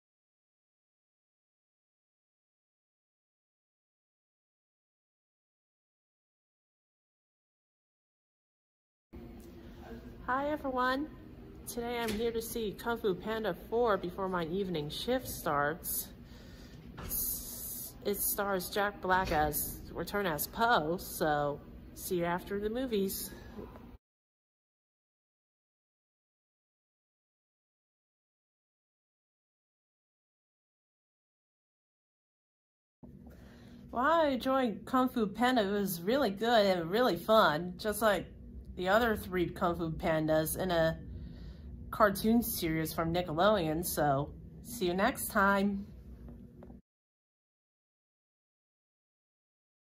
Kung Fu Panda 4
Yesterday I went to the movies again to go see Kung Fu Panda 4 before my shift started at Regal Cinemas, and it was really good and really fun.